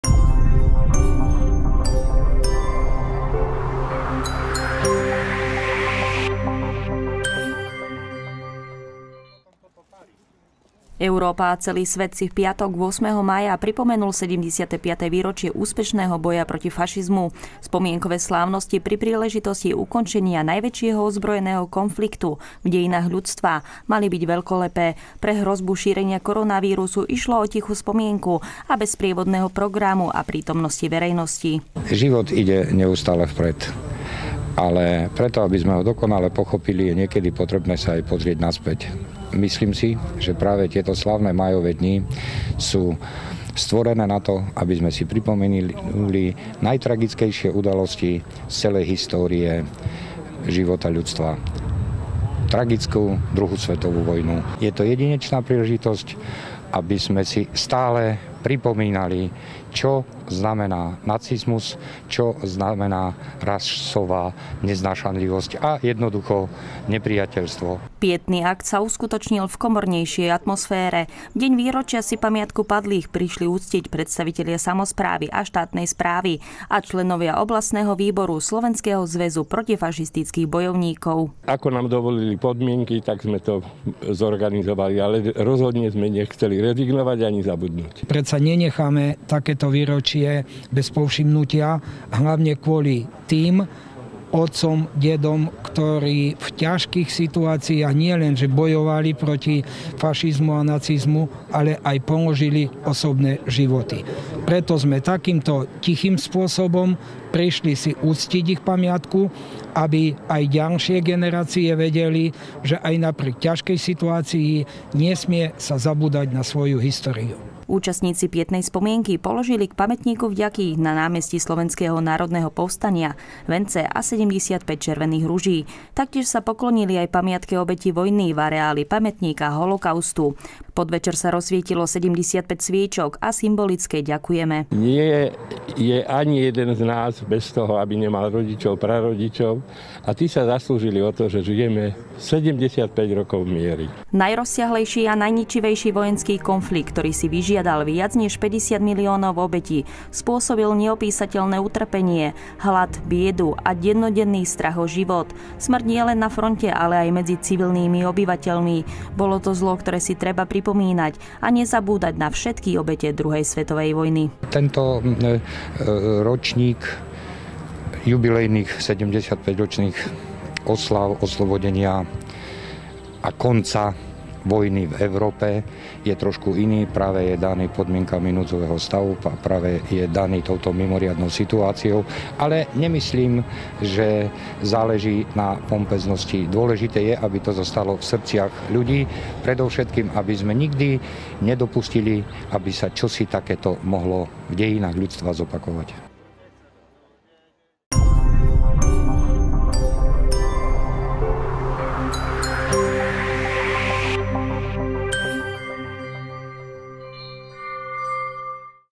75 rokov v mieri
Európa a celý svet si 8. mája pripomenul 75.výročie úspešného boja proti fašizmu. Pietny akt sa pri Pamätníku vďaky uskutočnil v komornejšej atmosfére. Išlo o tichú spomienku.

redaktorka: Jana Hnatková
kamera: Alica Hurtuková, Milan Marcinov ml.